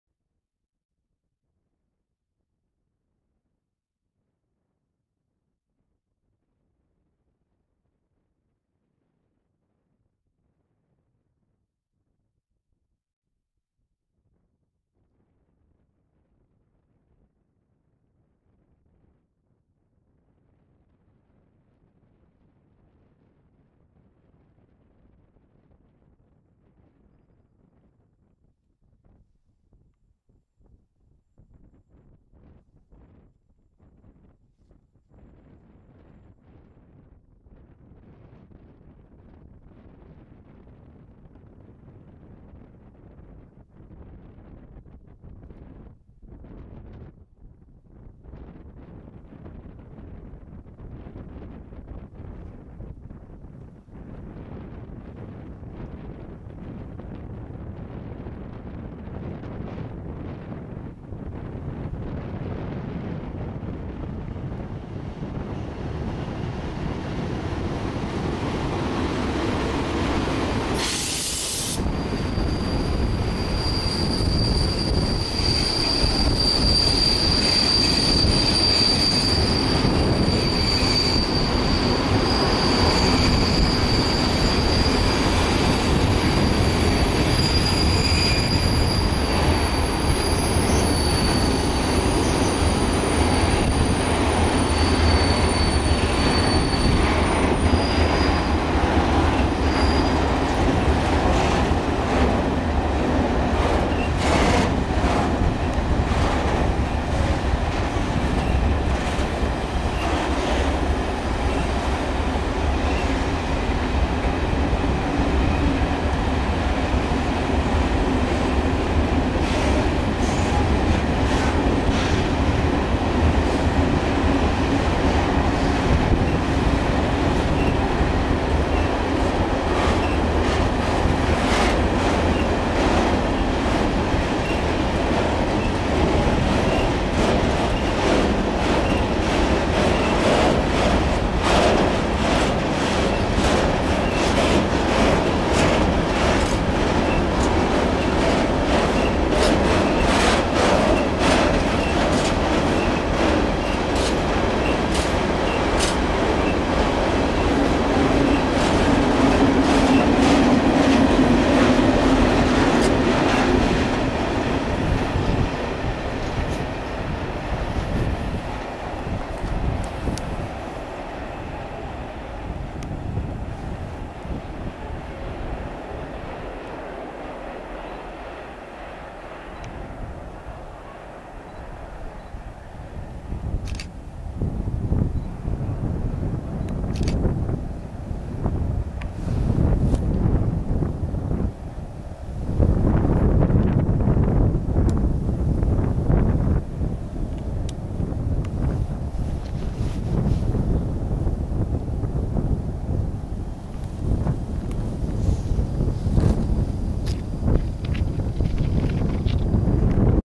Skoda 163 z tallbotami na łukach koło Charsznicy
Na filmie Skoda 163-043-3 z tallbotami na linii 62